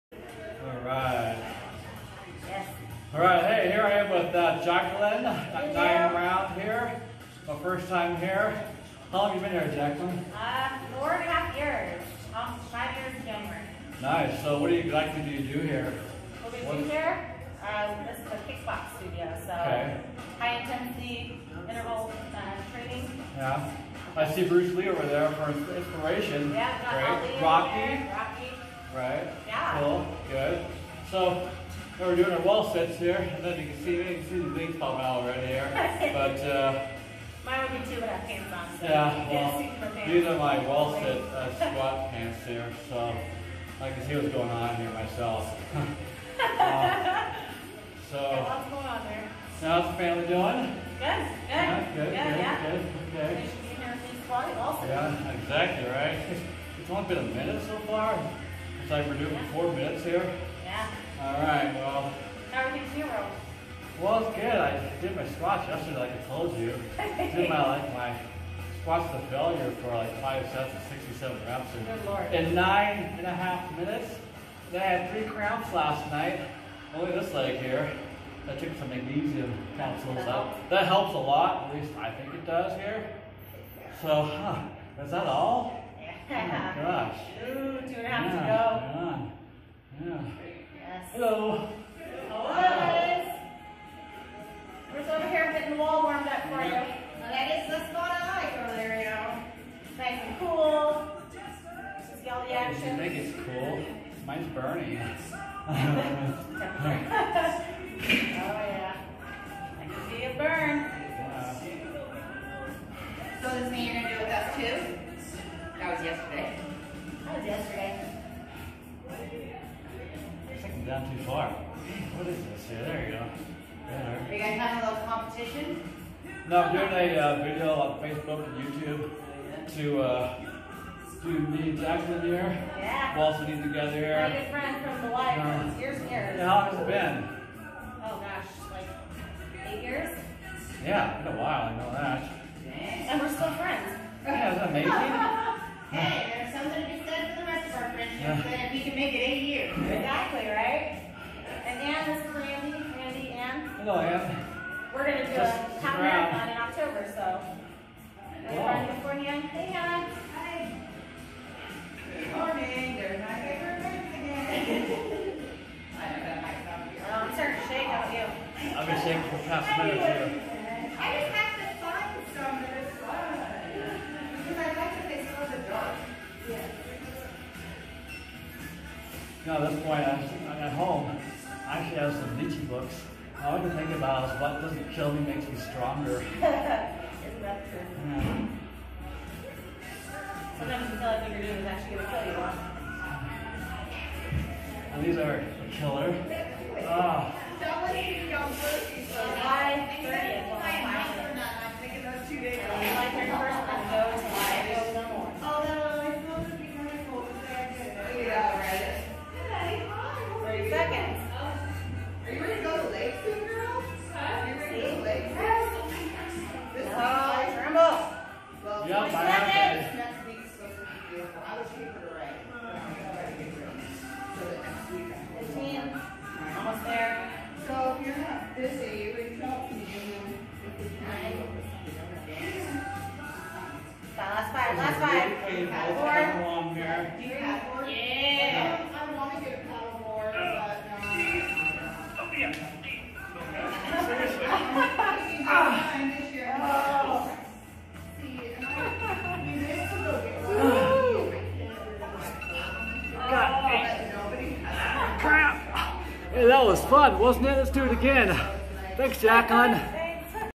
How to Wall-Sit for 4 Minutes (raw and uncut) 🔥
I hadn't seen Jacqueline in years. She invited me to her gym to video a wall-sit together. The previous day I did 5 sets of squats to failure on each set in about 10 mimutes. My quads were already sore and tight. 😲 But what the heck, let's do this! 💪

How to wall-sit is described more in my book, The Fitness Mindset. See below.

I began doing wall-sits shortly after the first COVID-19 Lockdown in spring 2020. I first did a 3-minute wall-sit and in two weeks progressed to 5-minute wall-sits after my seventh wall-sit. In six weeks and after my fourteenth wall-sit I worked up to 7-minutes.

My usual routine, however, consisted of 4-minute wall-sits of 4-6 sets with 2-minute box steps intervals between each set for active recovery.

A wall-sit is essentially a static negative squat. A 3-second wall-sit is equivalent to doing one body weight squat: 2 seconds down, 1 second up. Thus, a 1-minute wall-sit is 20 squats in 1-minute.

Are you up for a 3 or 5 or 7-minute wall-sit challenge? Wall-sits challenge the mind to endure the burn where results literally begin to appear. Wall-sits build strength, muscular endurance, and strength of mind.

Wall-sits are among the hardest exercises I have experienced that demand so much fortitude to constantly endure the burn 🔥 for a prolonged length of time. What doesn’t kill me, makes me stronger, right? Stay the course and focus!

I have observed a lot of people doing wall-sits with their feet out in front too far past their knees, and not rather under the knees to actually get into a squat position to feel and stimulate the quads. That is to say, the toes should be kept in line with the knees and ideally placed directly under them (not forward where the toes are in front of the knees and further from the wall) like a typical barbell or body squat. Granted, modifications are necessary but not at the expense of not feeling the quad muscles of a wall-sit.

People who put their feet out in front past their knees gives them the "illusion” that they did a 3 or 4 or even a 5-minute wall-sit when in fact they did not due to failing to get into a squat position to actually feel the constant burning sensation of the muscle fibers running up and down the quads.

Even when a person is wall-sitting near parallel, if the feet are out too far in front, it can cause the wall-sitter to use the floor with their feet to press their back against the wall to keep “wall-sitting.” Be honest with yourself and practice good form with modifications that makes the exercise both safe and effective for you.

Note: This wall-sit video was originally performed on July 24, 2020 and republished on YouTube August 10, 2021.